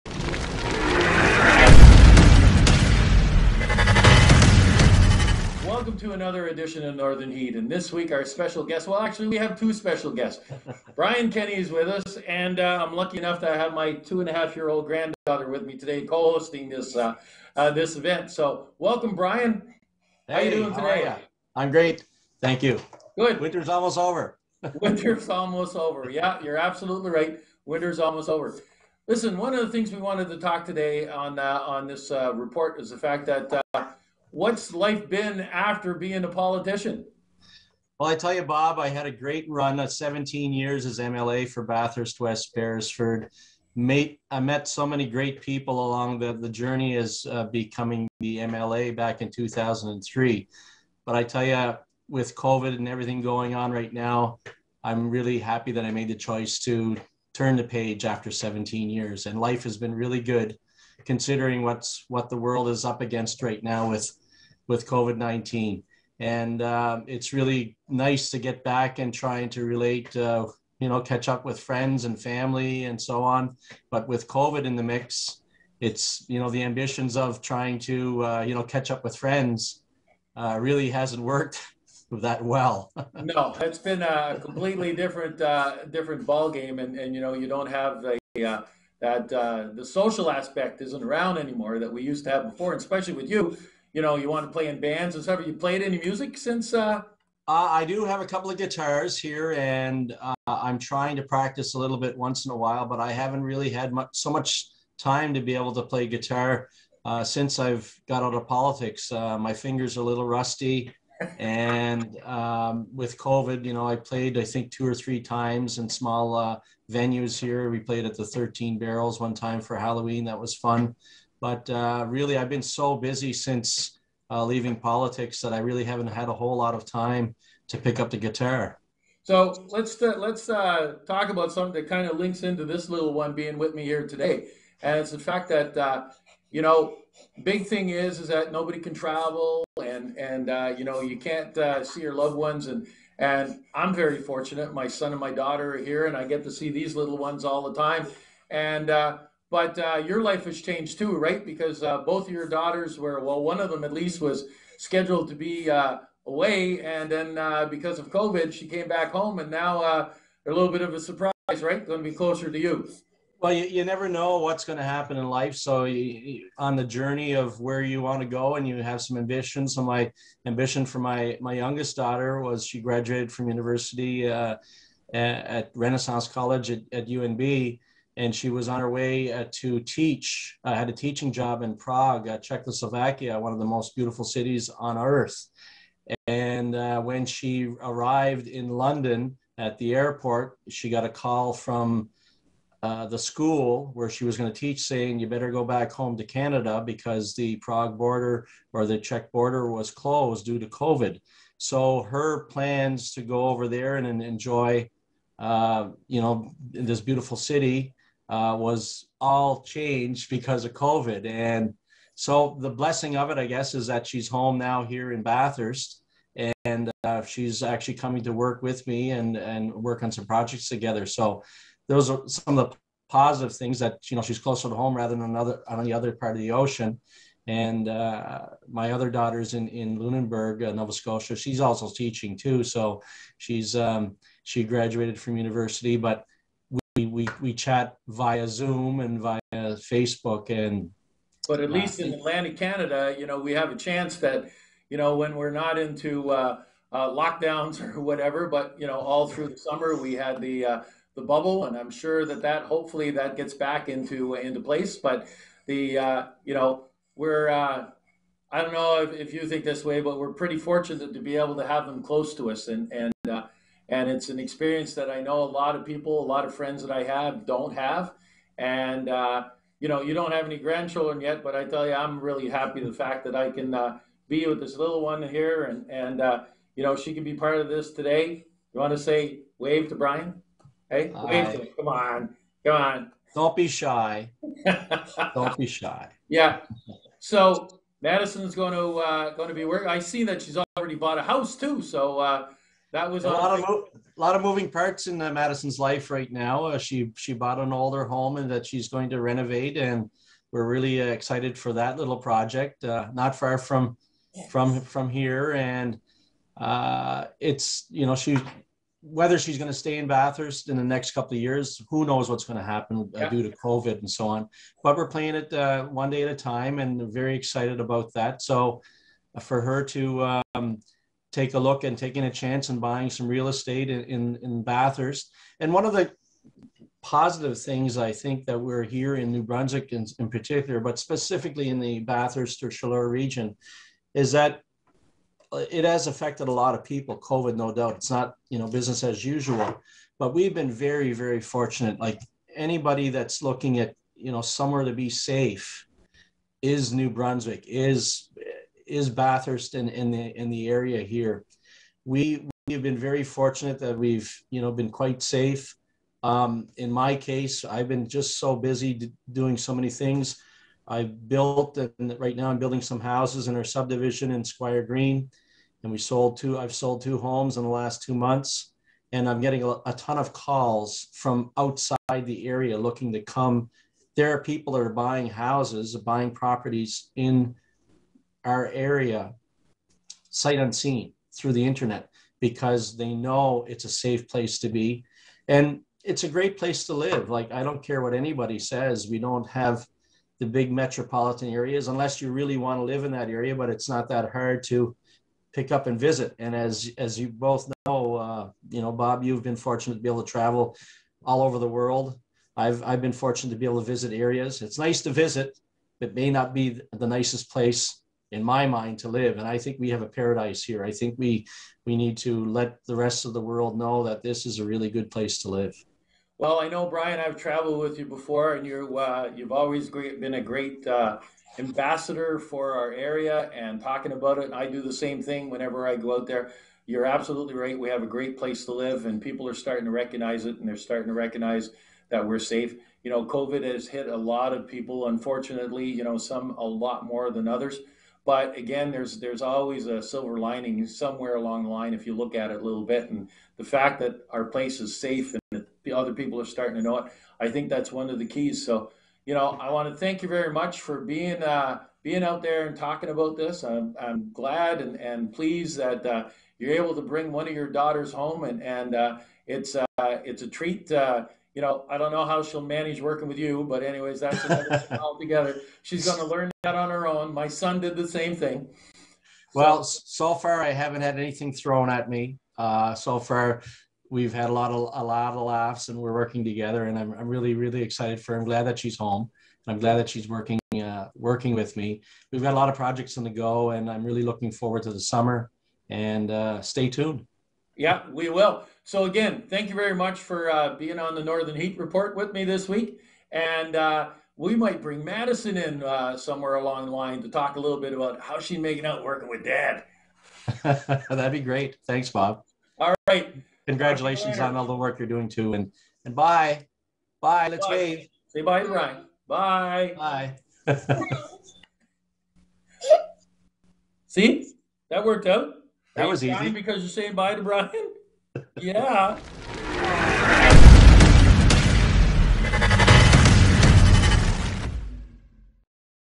The Northern Heat Report Brian Kenny
This weeks Northern Heat Report focused on some of the silver lining elements of COVID.  Our special guest Brian Kenny talks about how life has been since he left government and having his daughter join him in his new job.